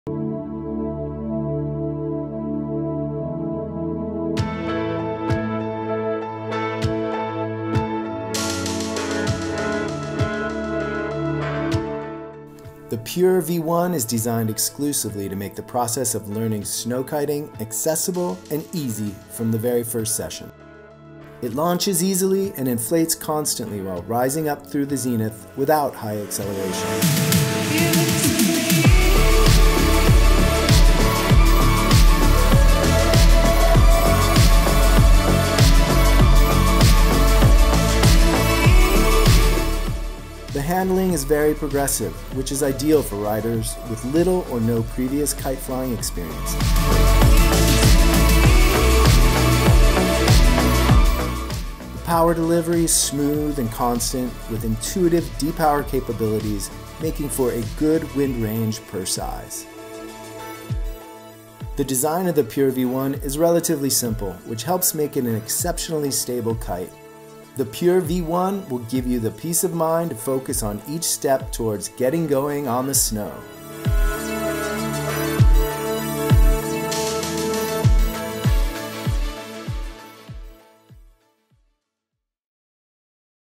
Ozone Pure V1 - A World of Opportunities
The Pure V1 is designed exclusively to make learning to Snowkite accessible and easy from the very first session. A cost effective de-power foil kite that is exceptionally stable giving you the peace of mind to focus on each step towards getting going on the snow!

The design of the Pure V1 is relatively simple which helps make it an exceptionally stable kite.

It launches easily and inflates constantly while rising up to the zenith without high acceleration. The handling is very progressive which is ideal for riders with little or no previous kite flying experience.

Power delivery is smooth and constant with intuitive de-power capabilities, making for a good wind range per size.

The Pure V1 will give you the peace of mind to focus on each step towards getting going on the snow!

PURE V1 CONTROL SYSTEM
We designed a new control system specifically for the Pure V1, incorporating a trimmer-less system to keep it clean and easy to use. The back leader line knots provide the trim option to increase power, turn speed and feeling on the bar.

The Pure V1 has been developed using relatively short flying lines, the 12m lines are the ideal length for new comers to safely learn and progress. Included in the package are 5m line extensions for an increased wind-window, providing more power for lighter wind days.

Self-landing the Pure V1 is simple and easy by pulling the brake handle between the back leader lines on the control system; the kite will stall down to the ground, when you are ready to launch again simply let go of the brake handle to re-inflate the kite.

Our time proven Front Line Flag Out is simple and effective. If you need to immediately stop all power from the kite, activate the release system and the Pure V1 will flag out to one front line, immediately stopping all power delivery.

PURE V1 HARNESS
The slim-line lightweight Pure V1 harness can be added to the package, setting you up with all you need for your first Snowkiting experience. The harness is fully adjustable to fit all shapes and sizes, both under or over winter clothing.

SNOW KITING TUTORIALS
We have created a free 4 step video series to guide you through the requirements to get up and riding: